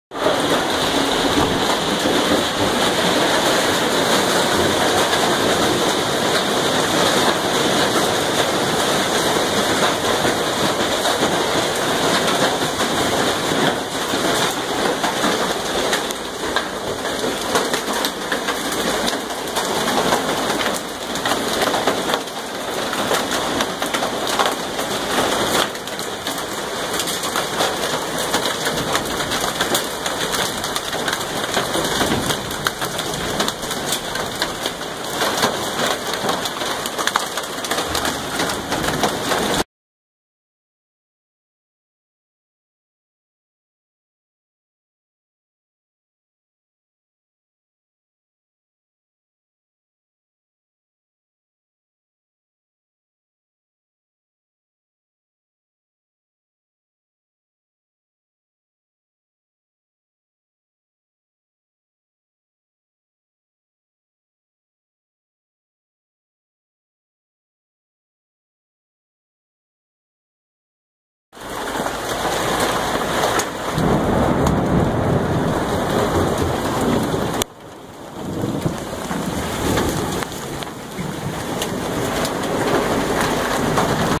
Composite video of hail in my back yard 05/12/2016.
Three videos including slo-mo of the hail falling.